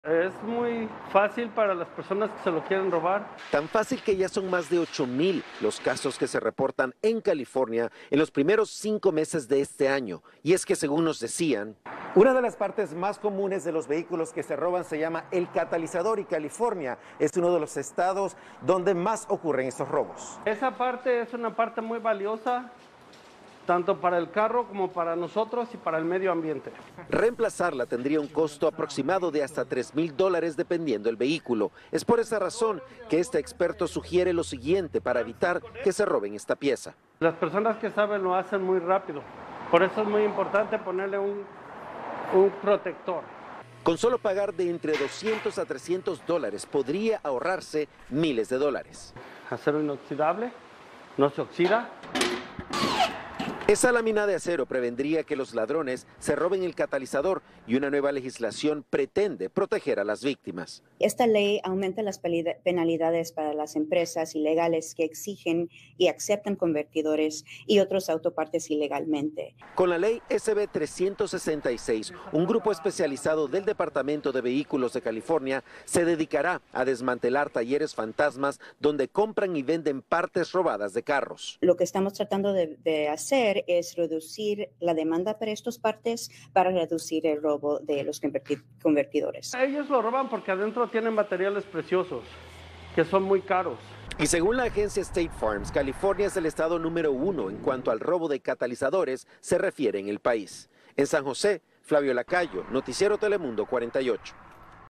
Mano dura contra el robo de catalizadores en California
Una nueva ley permitirá aumentar las penalidades para las empresas ilegales que exigen y aceptan convertidores y otras partes de auto ilegalmente.